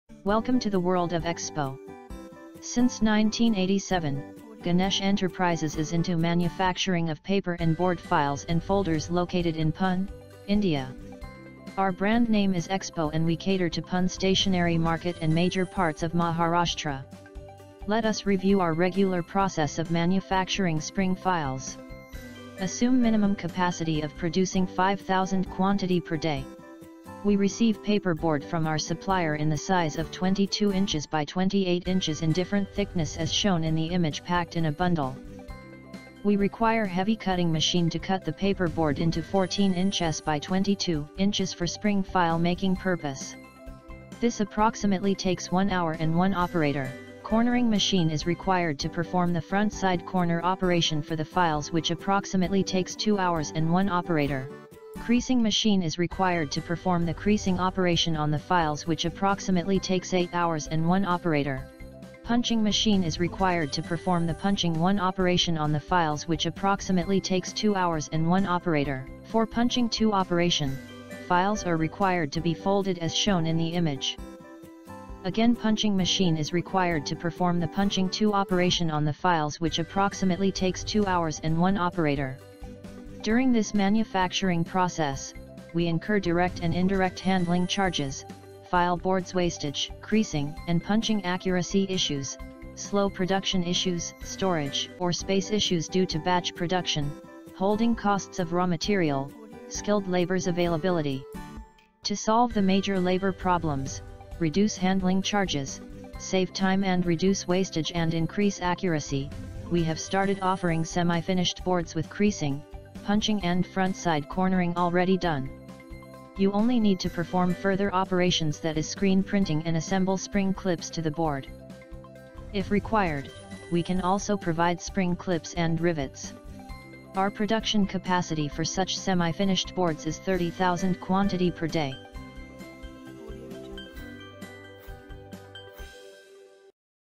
Semi Finished Spring File Board
This video is for the following customer categories-

- If you are a notebook/ files manufacturer/ plastic files manufacturer or any other paper & board products manufacturer and are interested in increasing product range, we have developed a semi- finished product for you which you can assemble at your place.
- If you are into exports and want to increase your product ranges, we are capable of handling huge volumes.
- If you want to outsource your current manufacturing due to labor/ quality issues, we can be your OEM supplier.